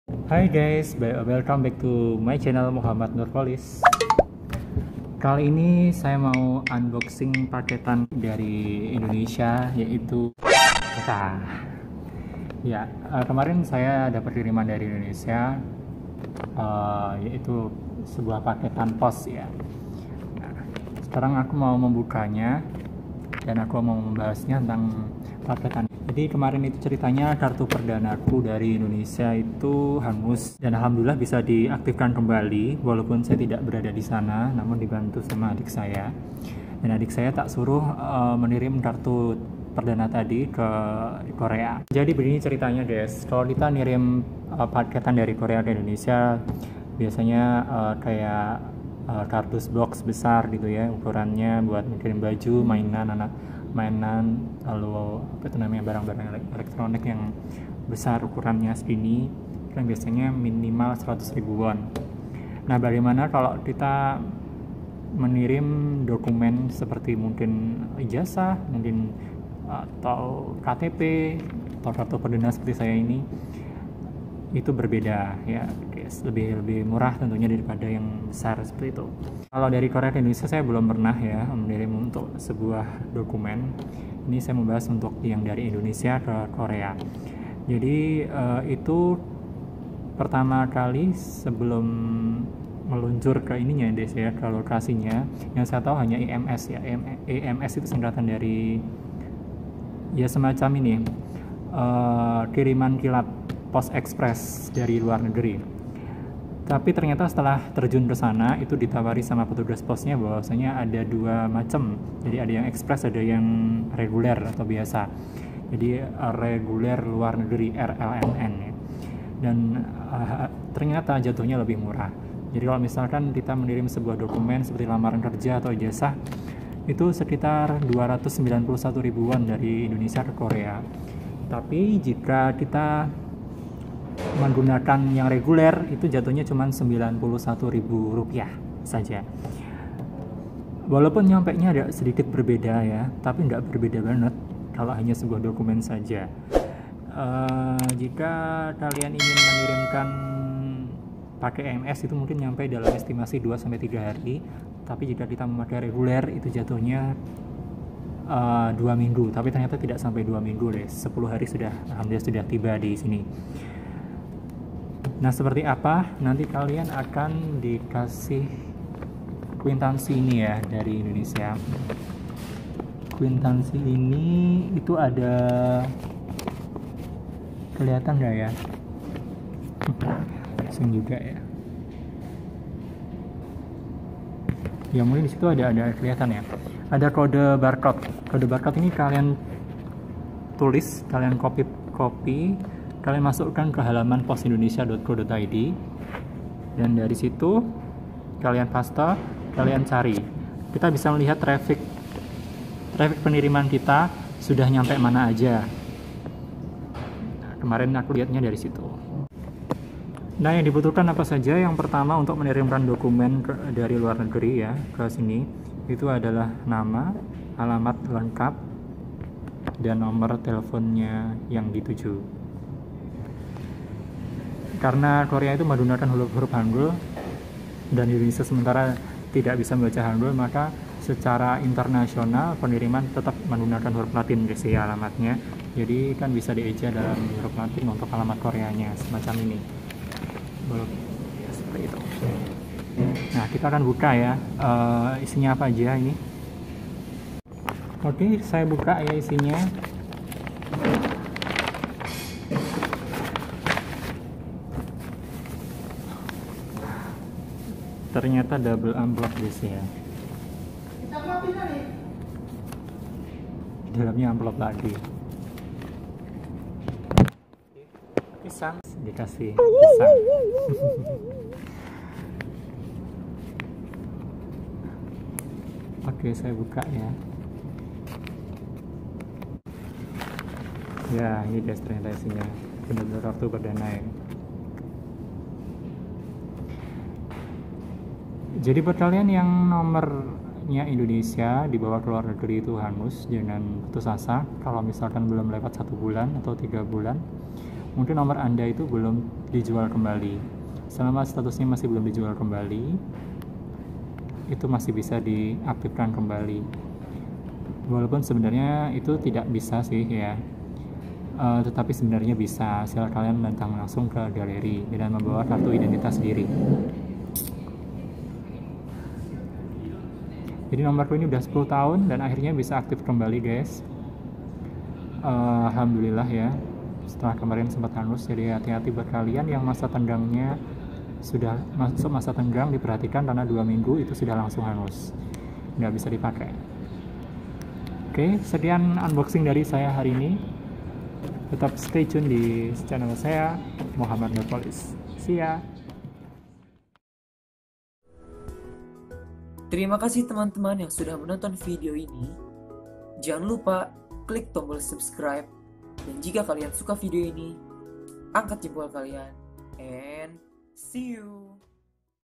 Mengirim Dokumen dari Indonesia ke Korea
Berapa sih biaya mengirim dokumen (surat penting, kartu HP, kartu kartu) dari Indonesia ke Korea?? Apa saja sih pilihan pengirimannya??